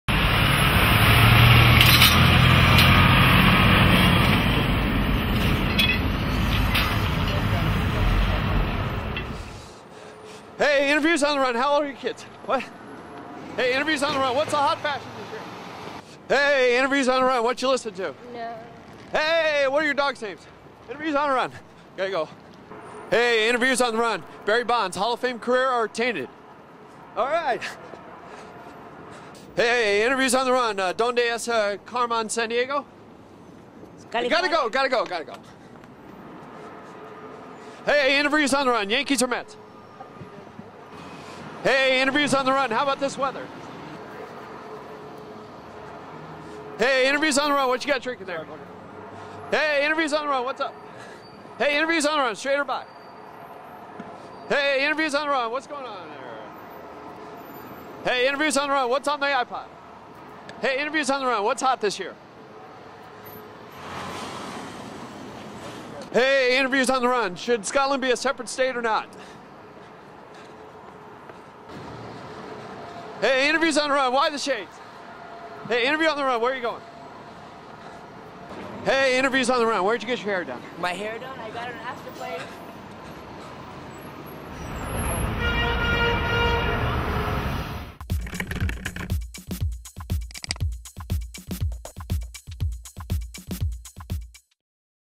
Bob Squad - Interviews on the Run
Bob Wiltfong takes comedy to the streets with a microphone, a lot of questions, and very little time.